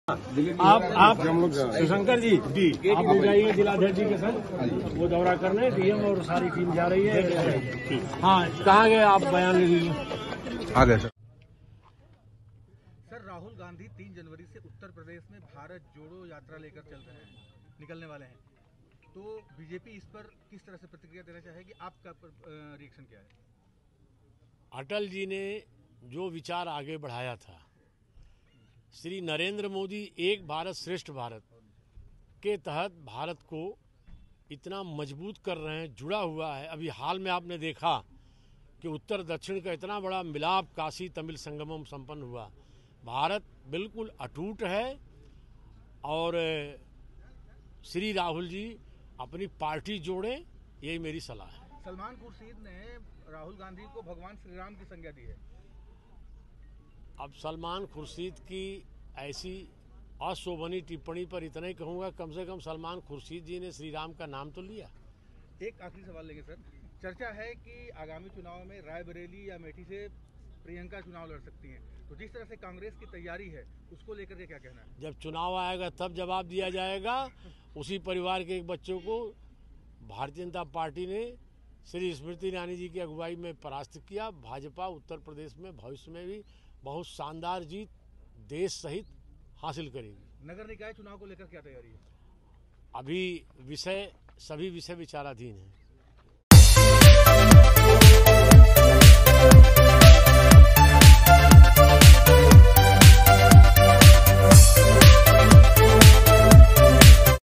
Chandauli में Mahendra Nath Pandey । Rahul Gandhi । Salman Khursheed पर दिया जवाब । JantaJanardan
- सांसद चंदौली डॉ_महेंद्र_नाथ_पाण्डेय का बयान

- राहुल गांधी की यूपी भारत जोड़ो यात्रा पर बयान- भारत बिल्कुल अटूट है राहुल गांधी जी अपनी पार्टी जोड़े*

-कहा- अटल जी ने जो विचार आगे बढ़ाया उसी को मोदी जी ने एक भारत श्रेष्ठ भारत के तहत काशी तमिल संगमम उत्तर और दक्षिण भारत को जोड़ा- महेंद्र नाथ पाण्डेय

- सलमान_खुर्शीद ने राहुल गांधी को श्री_राम की संज्ञा दी इसपर बोलें- कम से कम सलमान खुर्शीद ने श्री राम को याद तो किया

- नगर_निकाय और लोकसभा_चुनाव पर बोले भाजपा यूपी में शानदार जीत भविष्य में हासिल करेगी